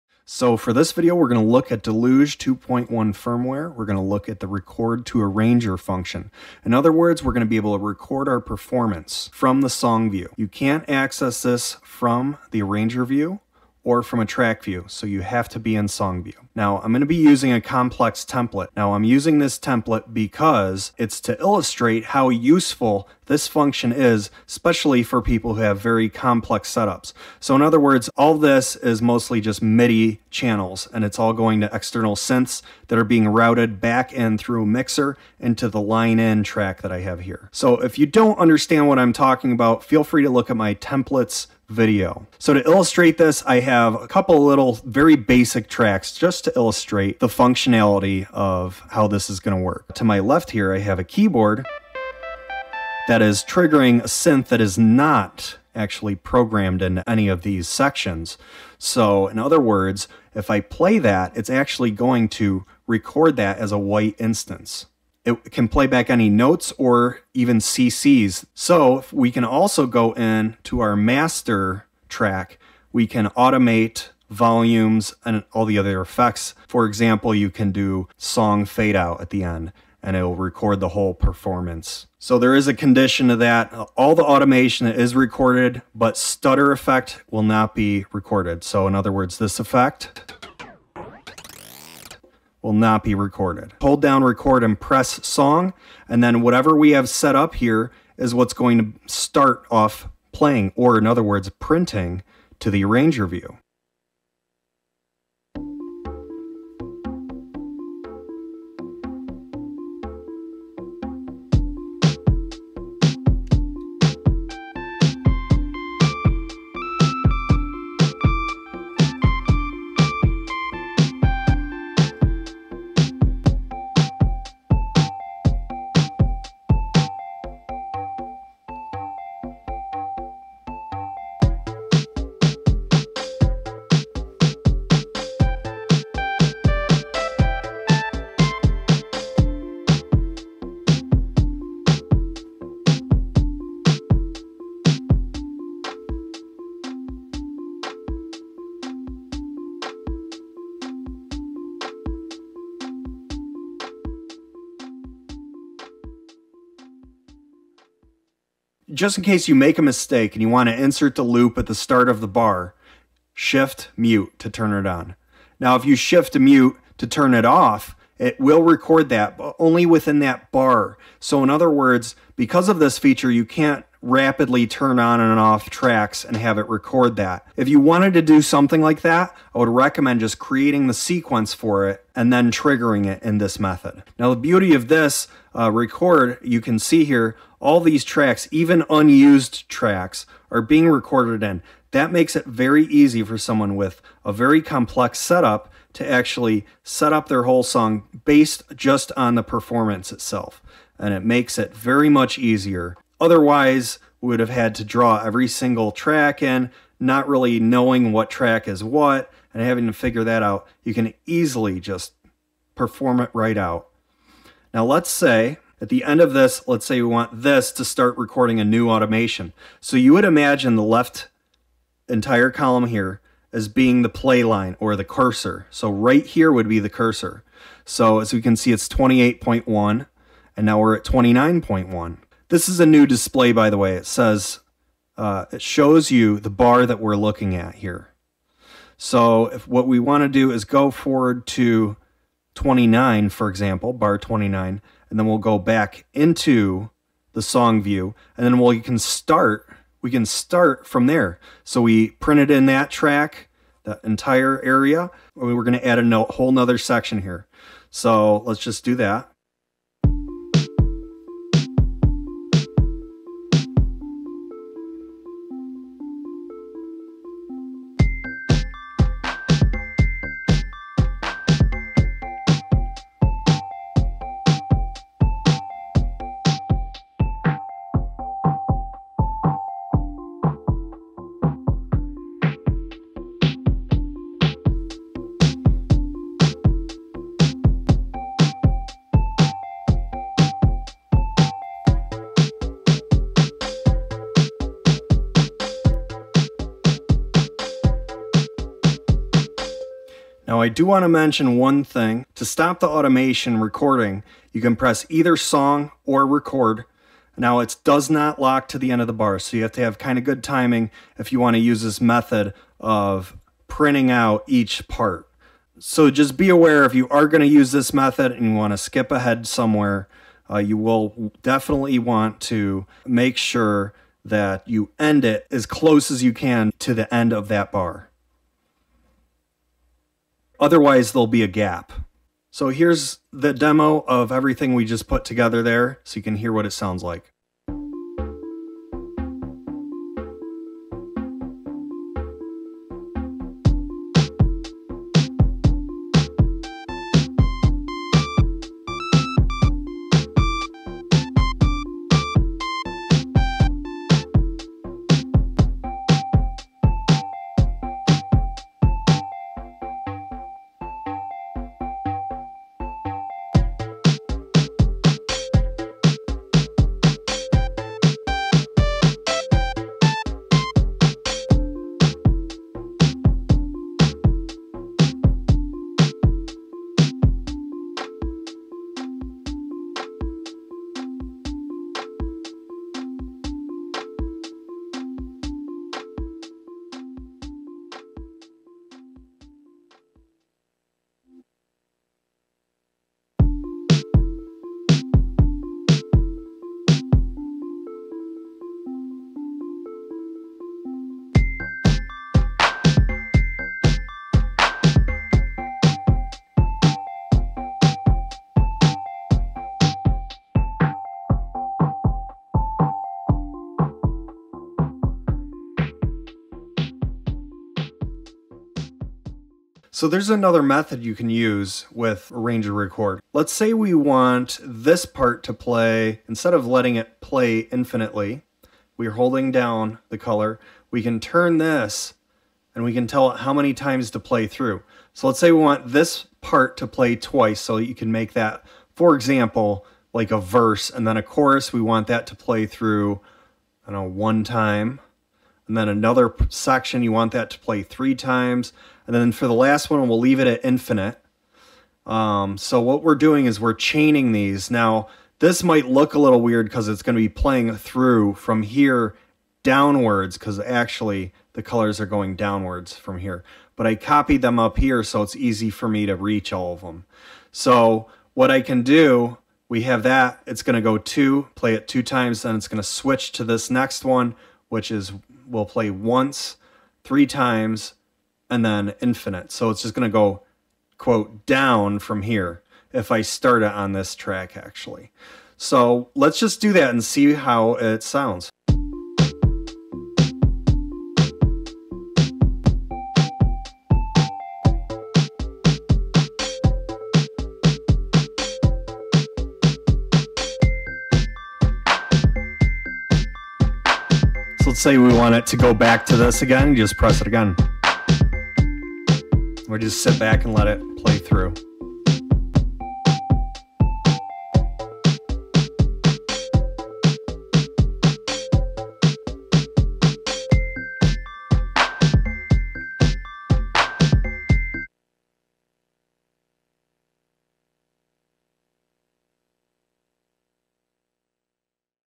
Record To Arranger (2.1)// Synthstrom Deluge Tutorial
This tutorial shows how the new record to arranger works on the new 2.1 firmware for the Synthstrom Audible Deluge. In this tutorial we are showing how easy it is now to compose tracks by performing them, even with a complex setup. In this situation we are using the following gear with the audio coming back into the Deluge using a line-in oscillator: Korg Emx1, Novation Circuit

Intro 0:00
Recording to Arranger Demo 1:33
Shift + Mute Function 2:39
Looking over Arranger 3:03
Adding To Arranger Demo 3:34
Stopping Arranger Record 5:00
Playing Arrangement Demo 5:37
Section Chaining w/ Record To Arranger 7:00
Chaining To Arranger Demo 8:08